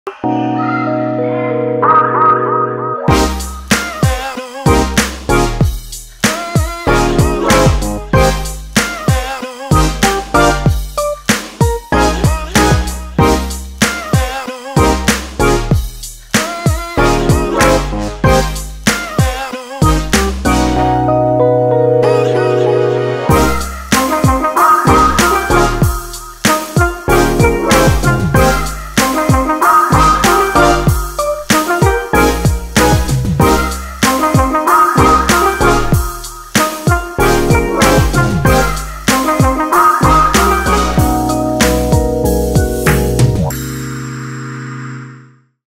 少女心粉色球池、網美野餐包廂、秒殺神級鬆餅，全家便利商店直接把VOGUE風格野餐日變成「極」Chill午茶派對，大玩食尚野餐趣
來VOGUE#風格野餐日，沒有#極鬆餅還像話嗎？討論度爆表的FamilyMart極鬆餅，如雲朵般膨膨軟軟的厚餅皮，秒成為野餐控X鬆餅控的心頭好～一日限定夢幻野餐打卡點、野餐包廂打卡點，讓人立刻沉浸在極鬆餅的粉紅泡泡球池裡，超幸福！